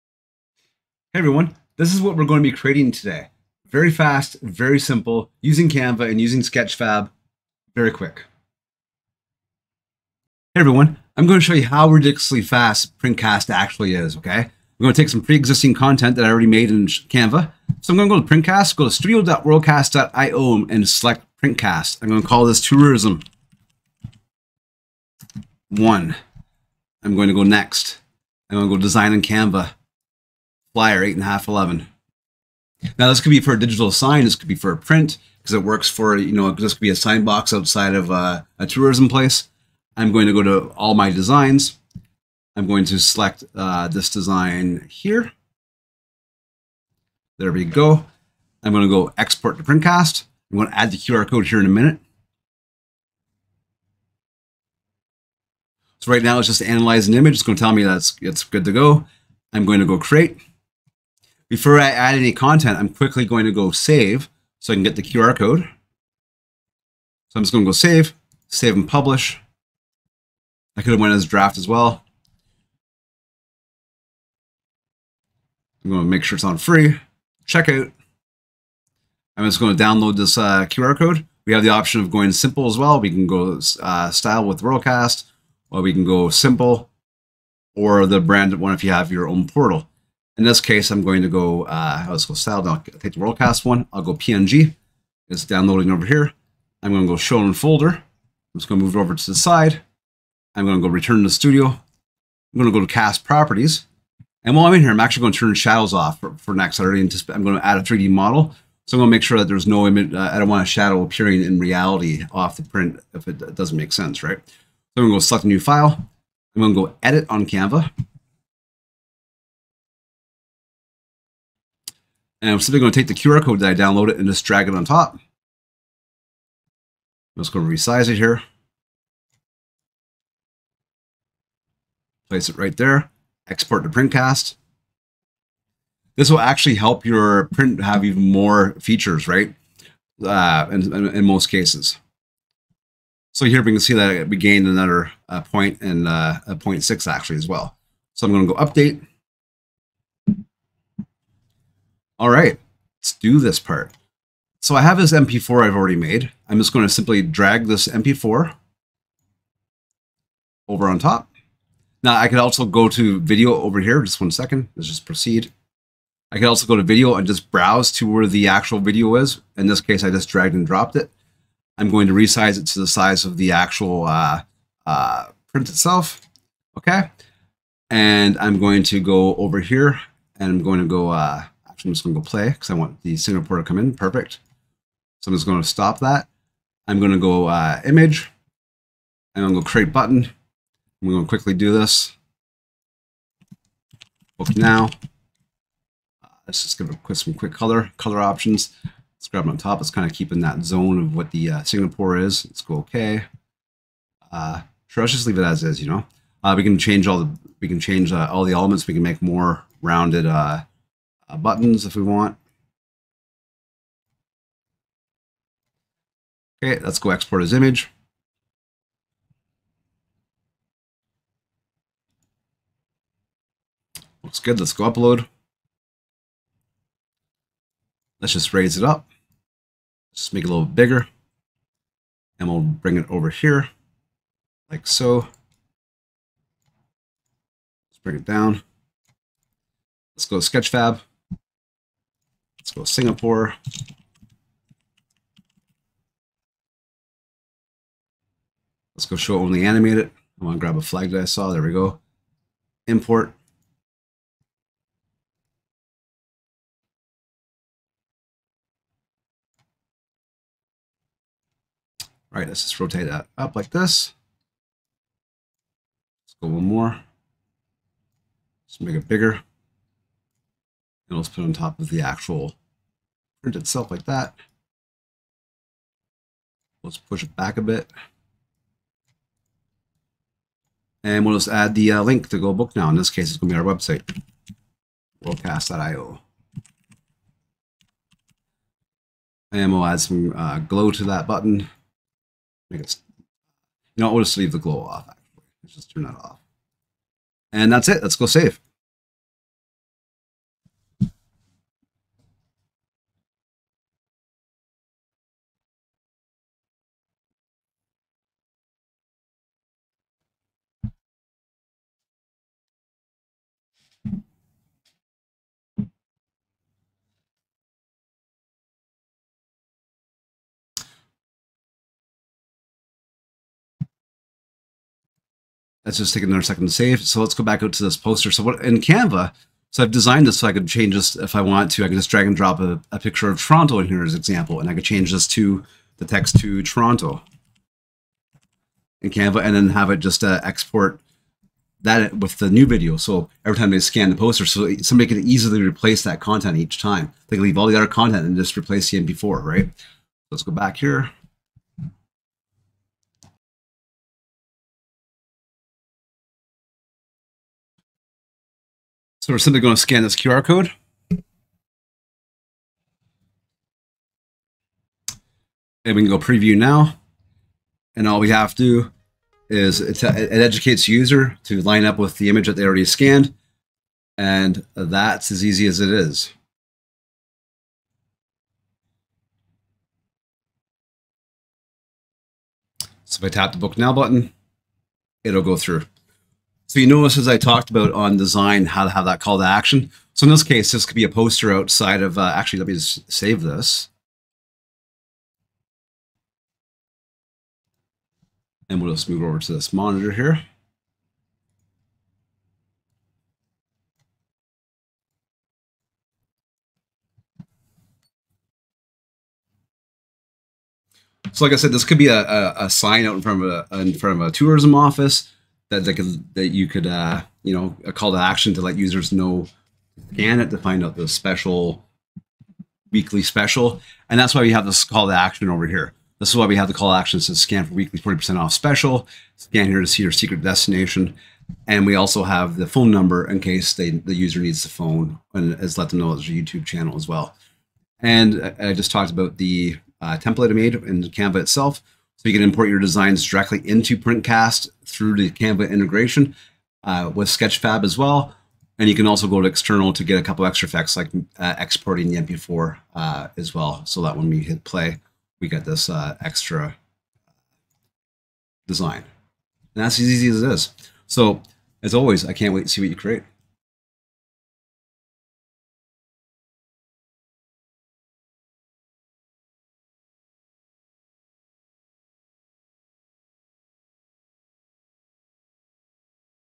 How To: WebAR QR Code Tourism Travel Interactive Print
In this tutorial we show how quickly an image can be made interactive using PrintCAST with a tourism/travel spin.

SINGAPORE BY SPACEXRACADEMY Available on Sketchfab

Get more Canva templates Free or with a Pro Account

Sign up for a free Sketchfab account to access free & premium 3D models